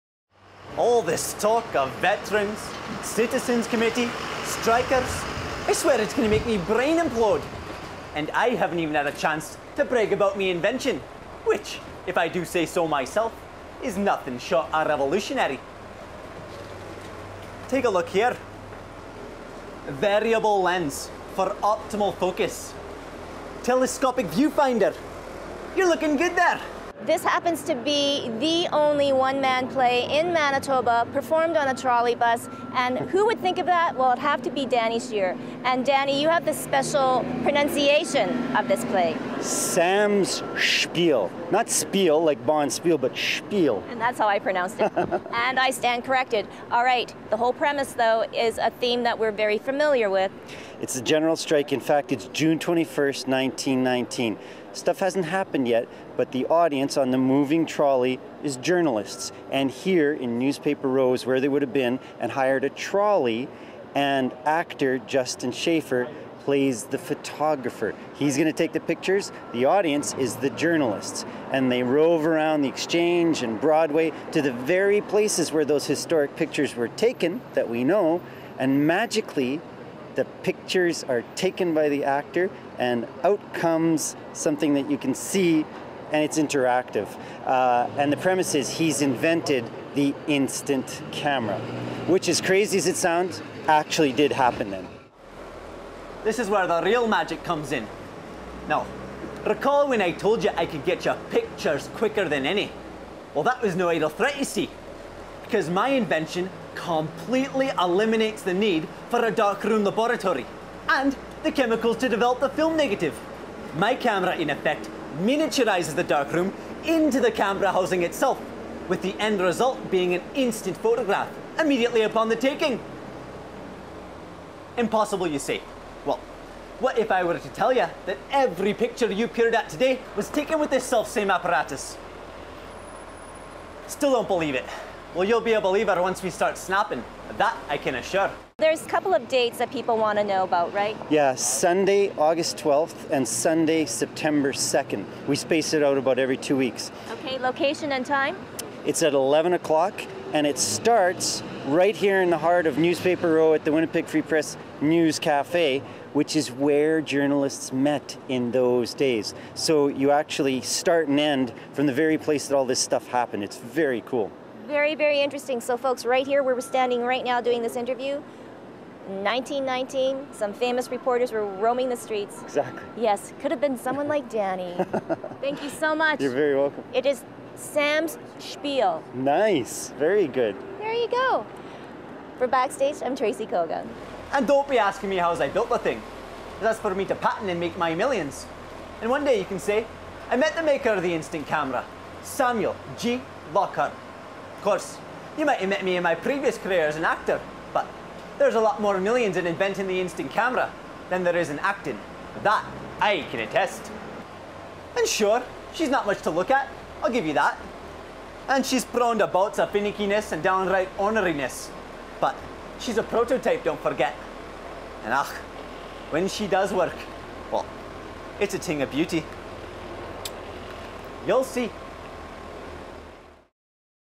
SamSpiel one-man play on the Trolley!
The continuing saga of the Winnipeg strike still goes on for palywright Danny Shur! Sam Spiel is his one-man version of a photographer caught on a trolley during the strike of 1919.